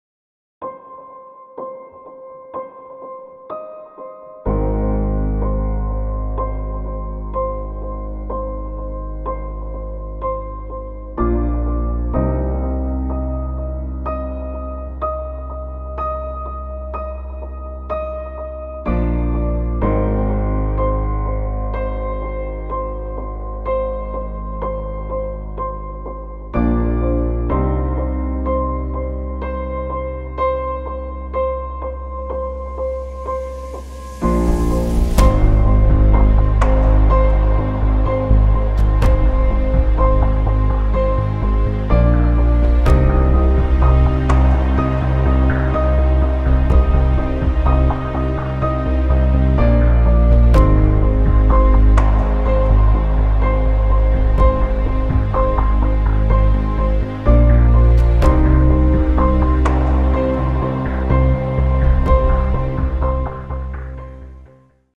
Spot fiestas Santa Cruz CALLE CABO septiembre 2023
Spot publicitario de las Fiestas de la Exaltación de la Cruz de la Santa Cruz de la Calle Cabo 2023. La Palma del Condado (Huelva)